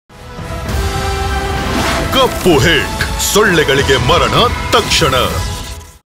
Kill mosquitoes instantly with Kala HIT | HIT India Kannada | 6s
When dengue spreading mosquitoes threaten your child, you attack and eliminate them. Kala HIT kills mosquitoes instantly and keeps your family protected, always.